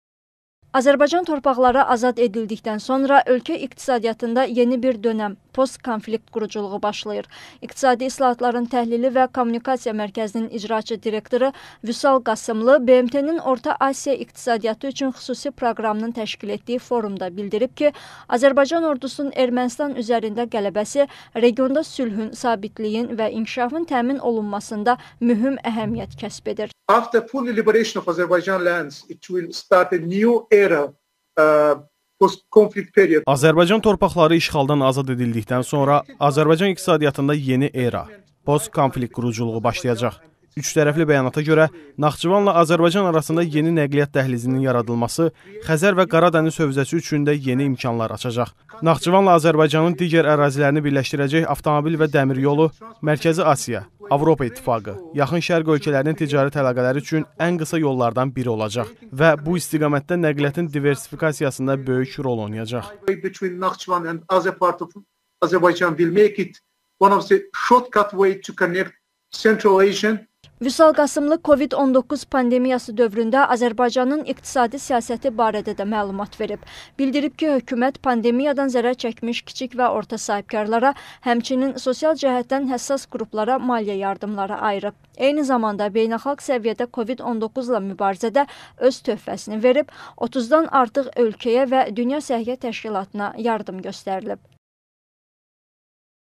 Vüsal Qasımlı “2020 SPECA İqtisadi Forumu”nda çıxış edib
İİTKM-in icraçı direktoru Vüsal Qasımlı "Covid-19 dönəmində sosial-iqtisadi bərpa üçün regional əməkdaşlıq" mövzusunda keçirilən konfransda çıxış edib.
Ətraflı REAL TV-nin efirində